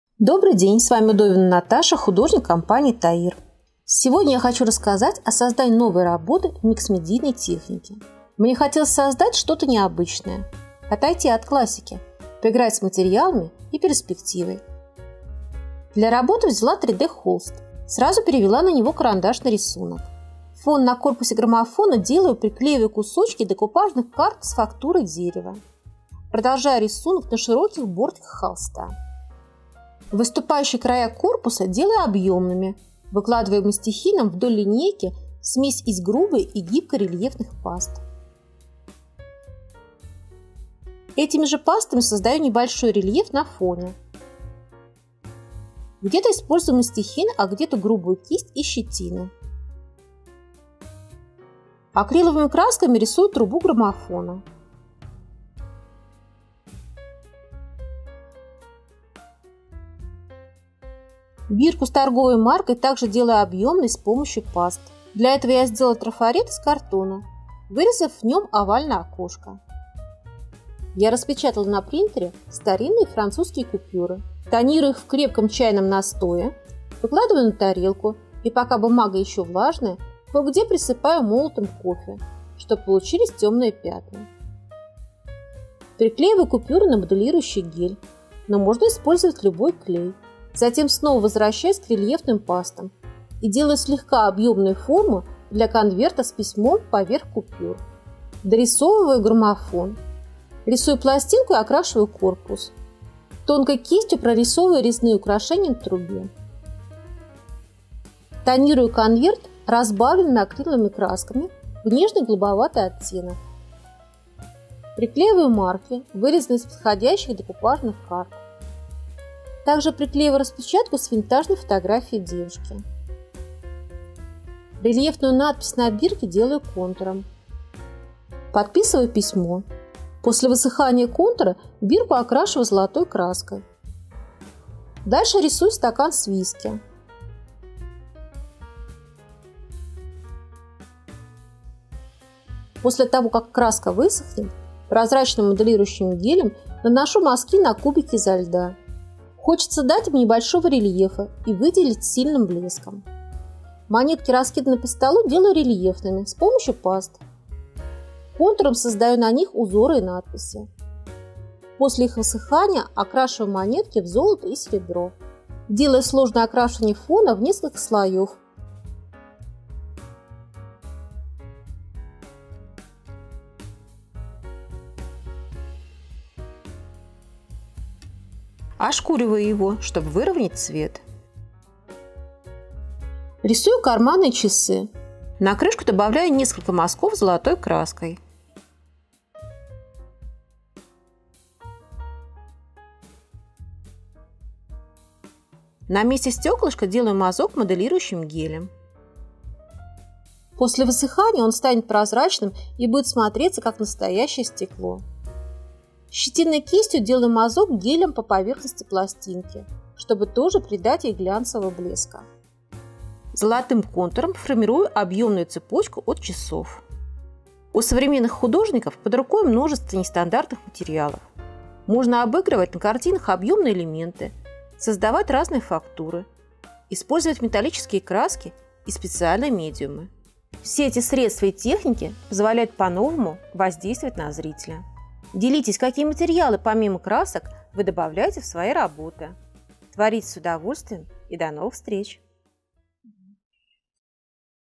Миксмедийная картина. Акрил и сочувствующие
Сложная картина, со множеством техник и материалов: рельефные пасты, кракле, коллажирование, тонирование .
Повторять не обязательно, а вот посмотреть используемые приемы мы настоятельно рекомендуем.

Все материалы, включая холст, нашего производства. Можно купить у наших партнёров в разных городах, на маркетплейсах, или в нашем интернет-магазине